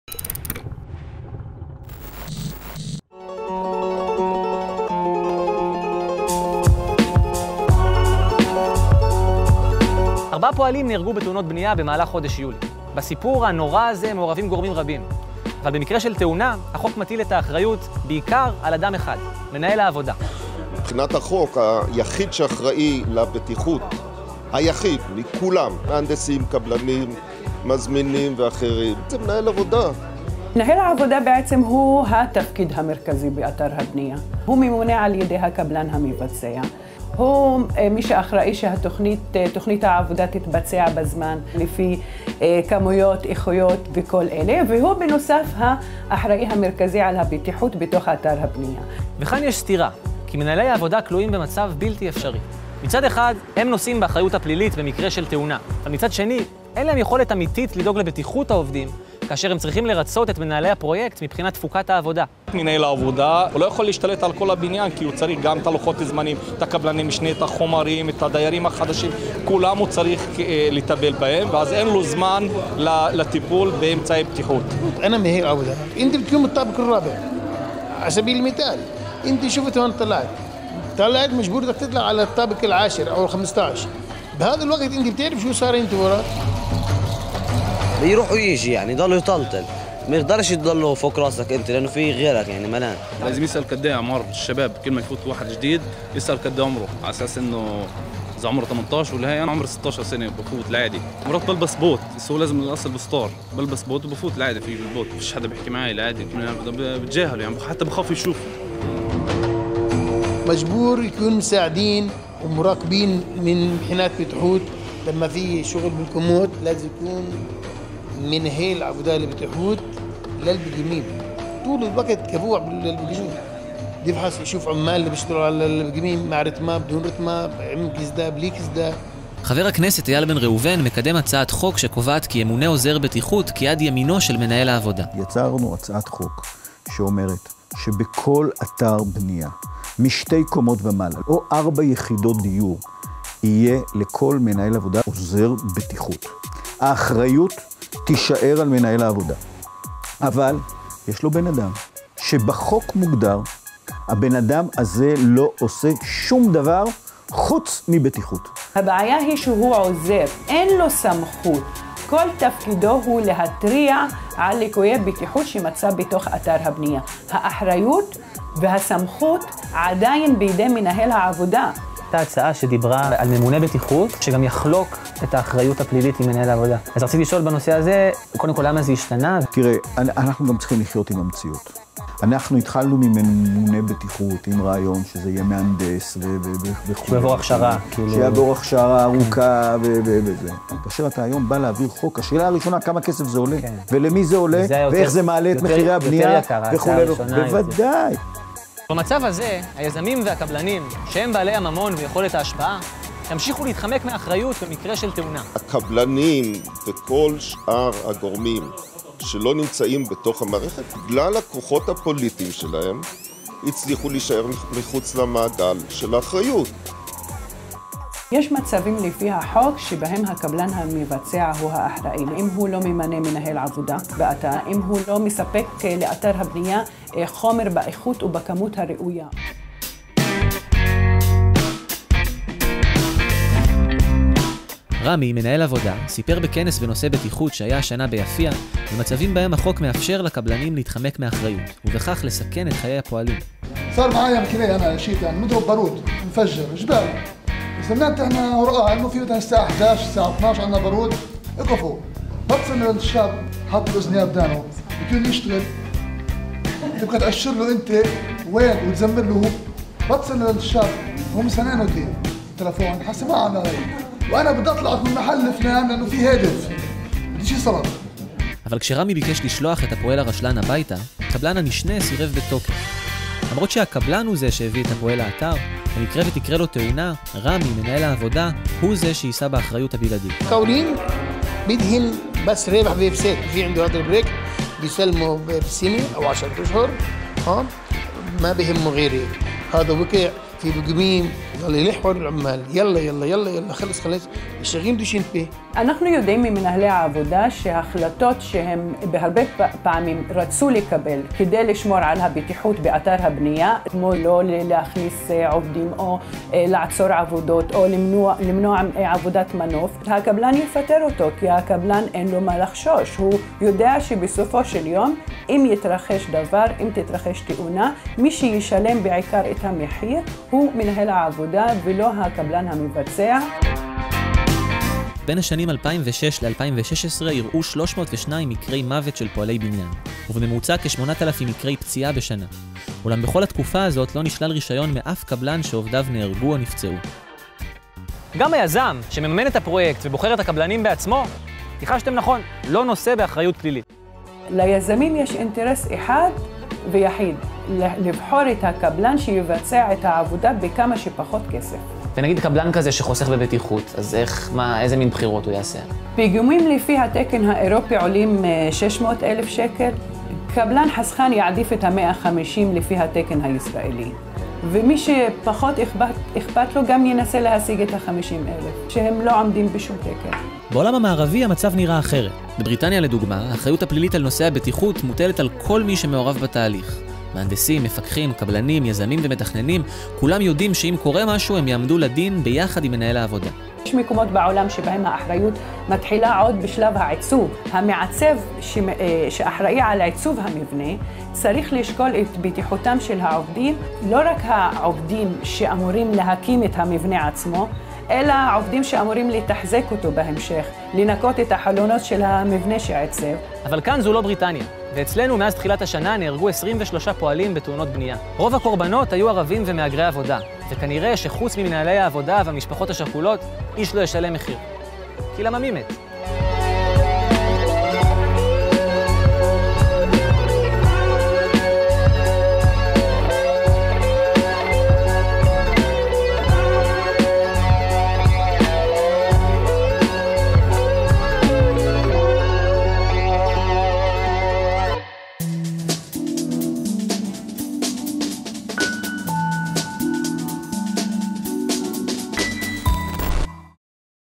A Conflicting Interest
The only person responsible for safety at the construction site legally and operationally is the foreman appointed by the developer or the contractor. But the foreman answers primarily to the person who appointed him for the execution of the construction project efficiently and under stress conditions. Should the foreman bear criminal responsibility in the event of an accident at the site? And why is the state not willing to appoint another worker that will be responsible for safety?
The article was produced as part of the "Nirtamim- Safety for Construction Workers" - a joint project of Worker’s Hotline, Ma'an Workers Union and the Social TV.

יש לכם טלגרם? מוזמנים להירשם לערוץ ולהתעדכן באופן שוטף בכל הפעיליות של הטלוויזיה החברתית